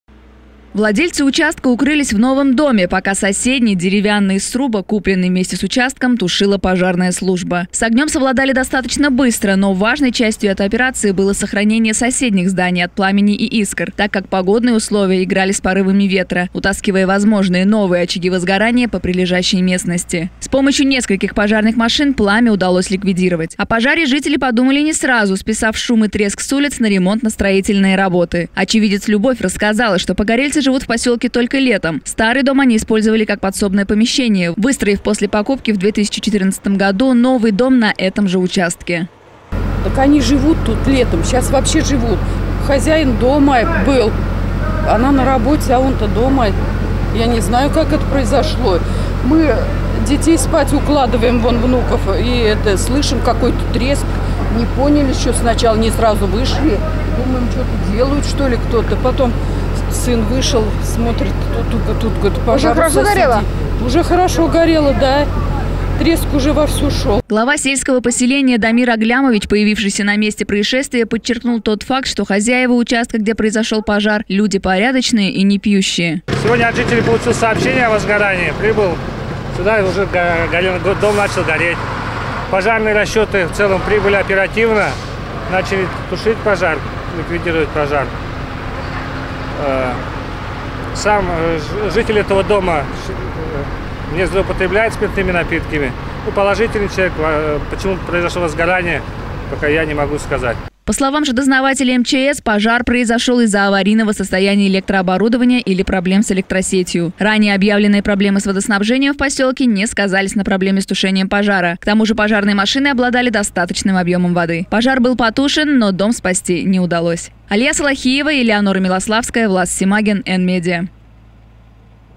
Очередная беда в селе Прости.
Буквально два дня назад наша съемочная группа снимала сюжет о нехватке питьевой воды в селе Прости. И вот еще одно несчастье. Накануне в селе загорелся деревянный дом. Наша съёмочная группа незамедлительно выехала на место.